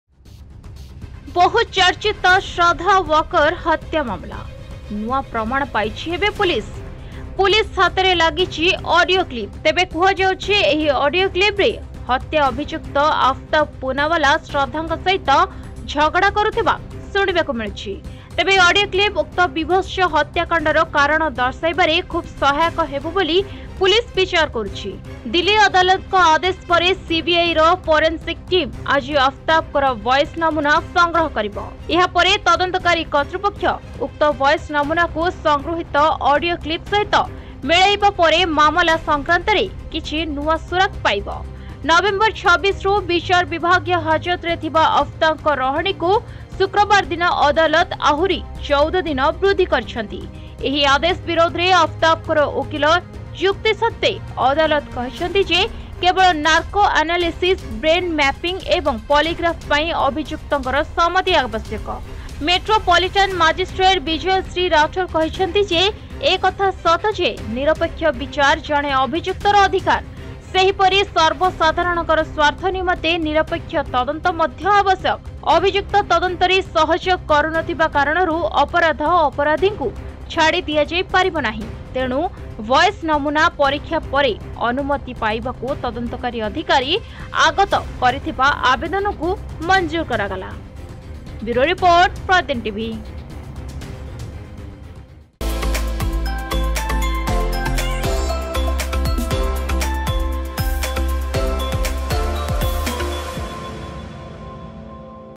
ଶ୍ରଦ୍ଧା ହତ୍ୟା ମାମଲାରେ , ନୂଆ ‘ଅଡିଓ କ୍ଲିପ୍’ ପାଇଛି ପୋଲିସ । Shraddha murder case ।PratidinTv
ଶ୍ରଦ୍ଧା ହତ୍ୟା ମାମଲାରେ , ନୂଆ ‘ଅଡିଓ କ୍ଲିପ୍’ ପାଇଛି ପୋଲିସ । Shraddha murder case ।